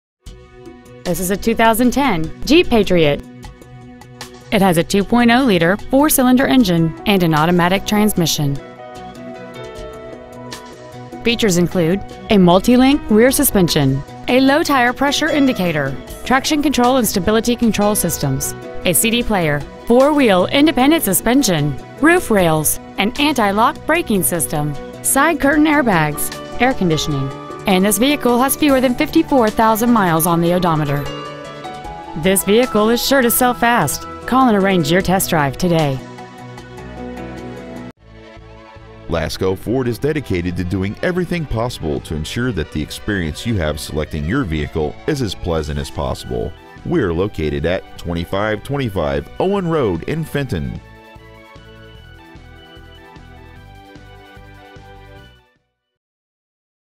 2010 JEEP PATRIOT Fenton MI
Year : 2010
 Make : JEEP
 Model : PATRIOT
 Engine : 2.0L I4 16V MPFI DOHC
 Trans . : AUTOMATIC
 Exterior : SILVER
 Miles : 53,719
 Interior : BLACK
 Stock : GP4587